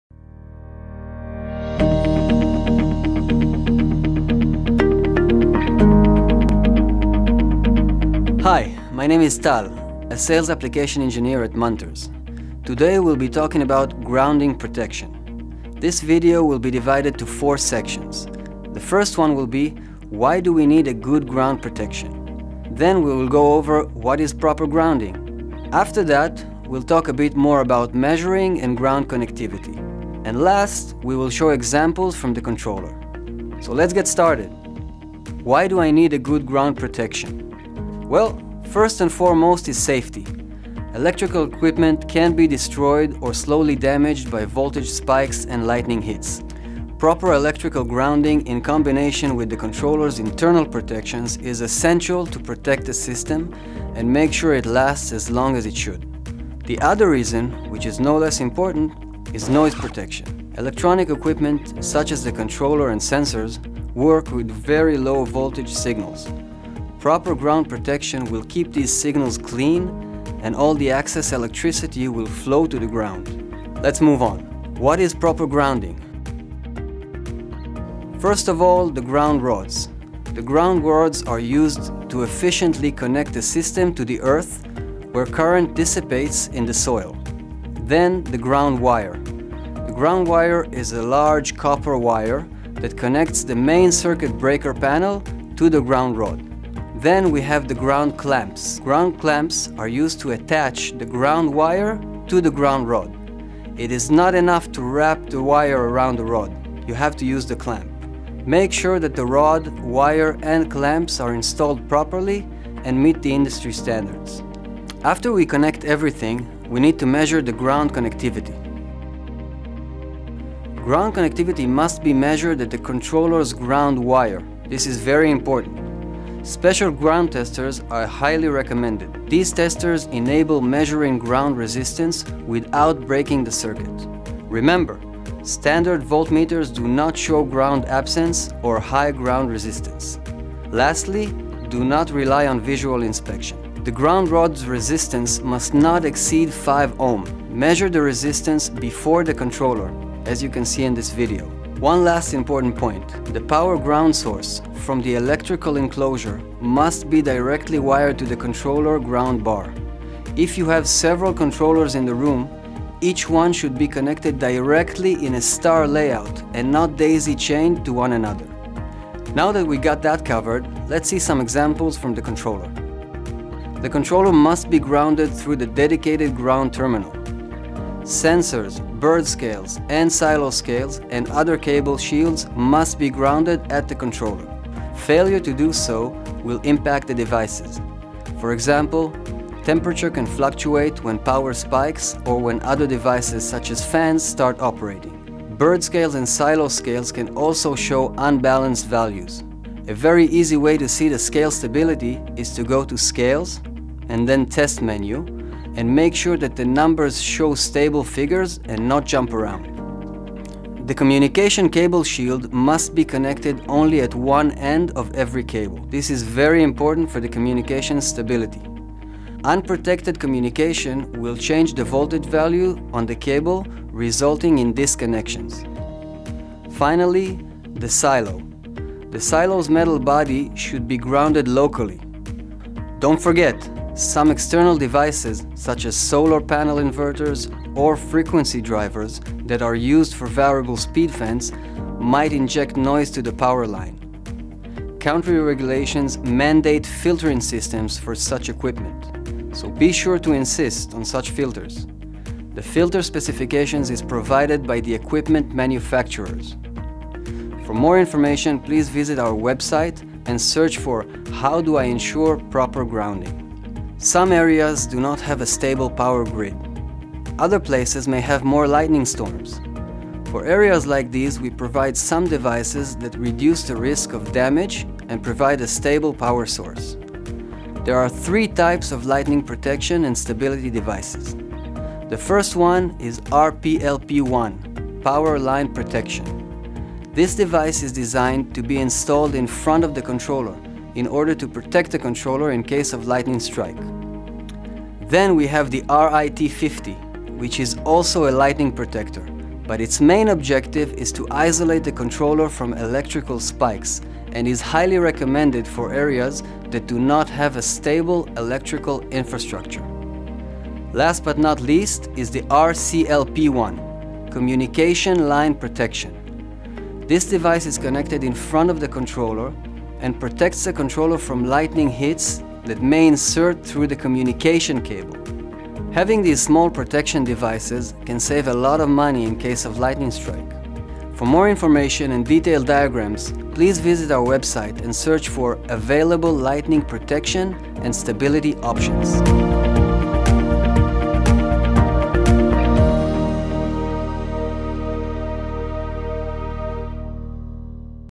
Grounding Protection
Why do we need good grounding? What is proper grounding? How to measure ground connectivity? Tal Cohen runs you through the basics.